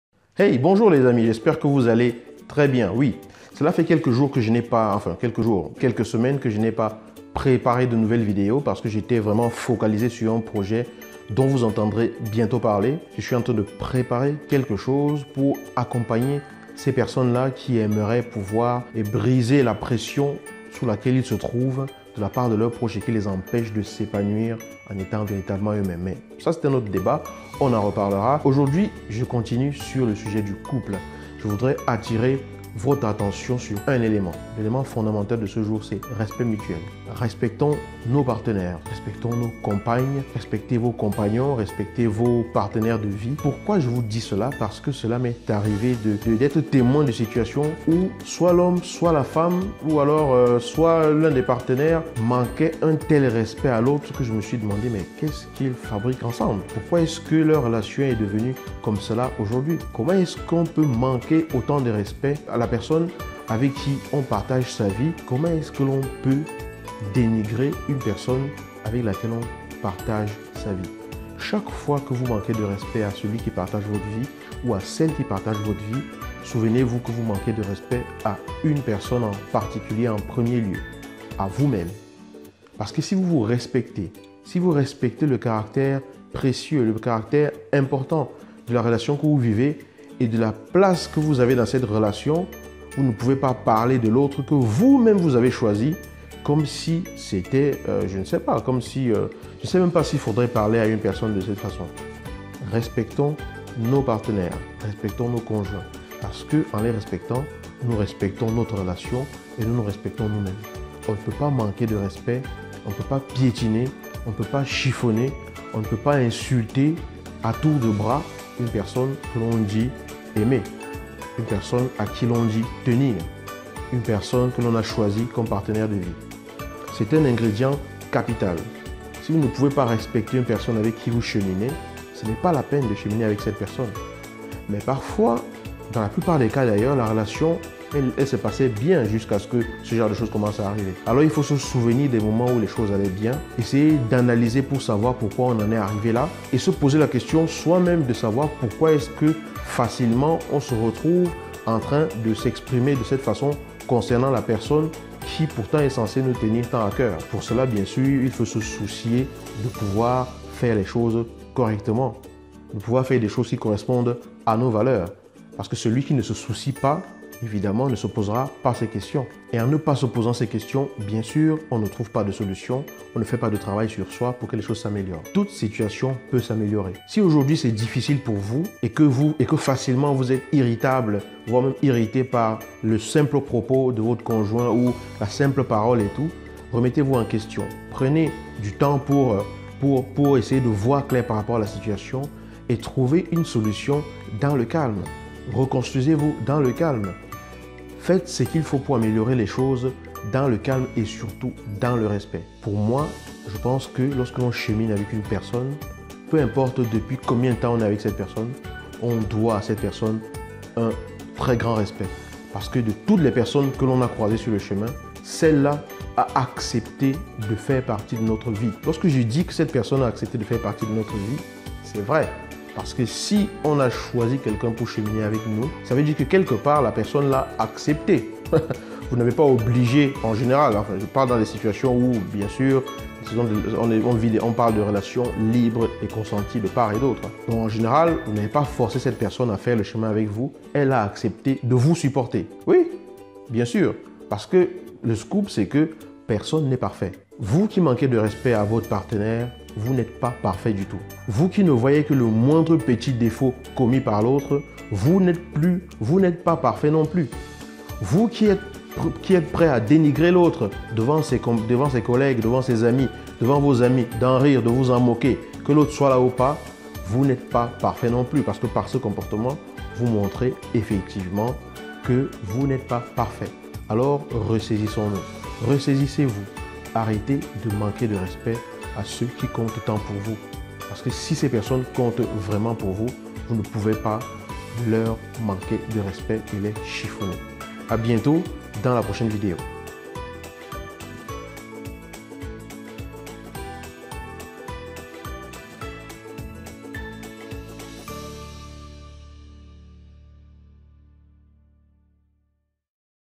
Manque de respect dans le couple
L'importance du respect dans un couple est à souligner. J'ai été témoin à plusieurs reprises de manque de respect entre partenaires, parfois dépassant ce que j'aurai pu imaginer. Pourtant cela semble normal pour certains, mais est ce vraiment normal